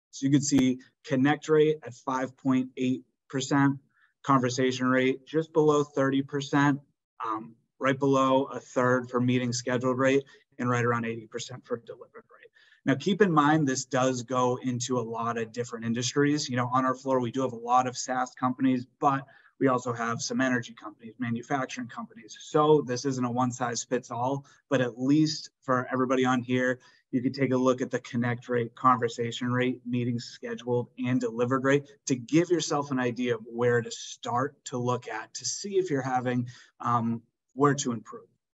Benchmarking your team's cold call conversion rates
This episode is the audio from our recent webinar: Steal ZoomInfo's objection handling framework. We were joined by Arely Brattin & Dana Zimmerman from ZoomInfo, and Gerry Casaletto from Reveneer.

▶︎ If you're a sales leader, check out the Outbound Accelerator